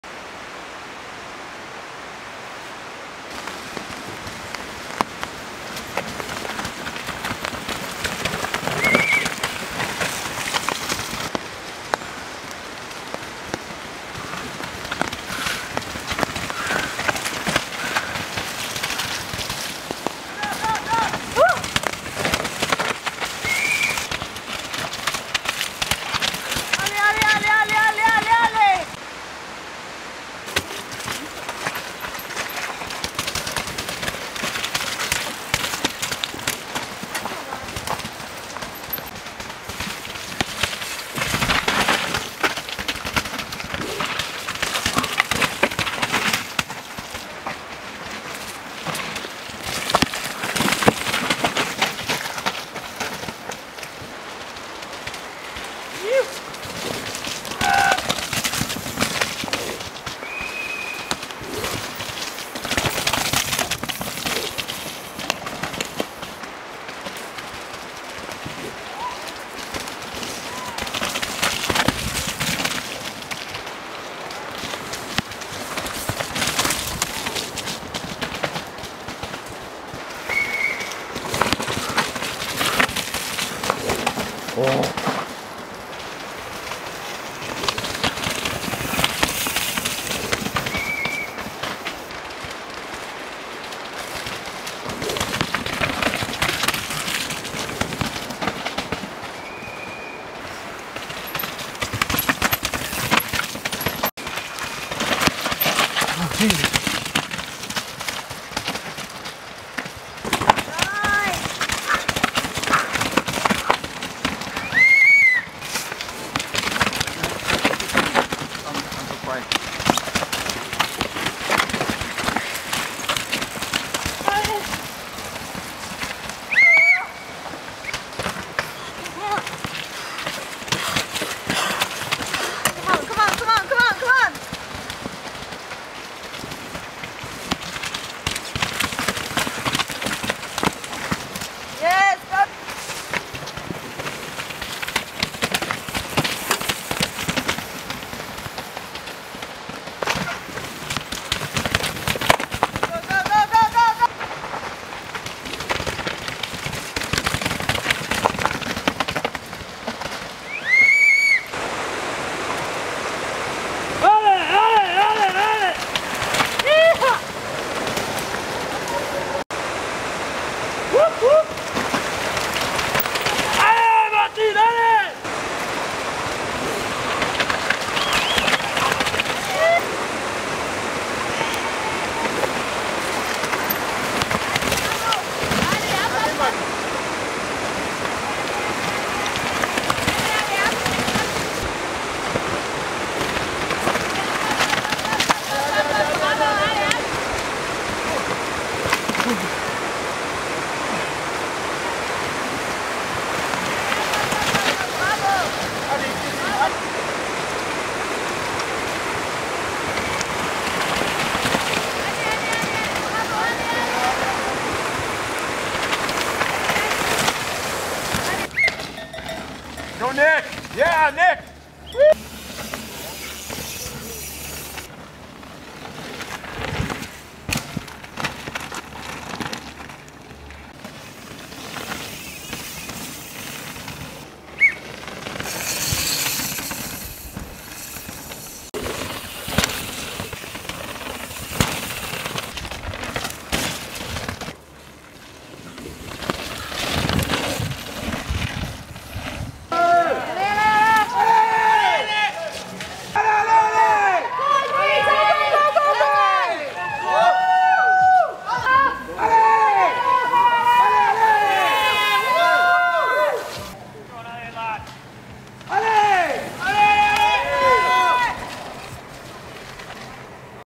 Racing On The Edge - EWS Zermatt 2020 Finals RAW
Despite horrific weather and stage cancellations the first EWS of the season still happened.

Results:

1. Jesse Melamed
2. Martin Maes
3. Theo Galy

1. Isabeau Courduier
2. Morgane Charre
3. Ella Conolly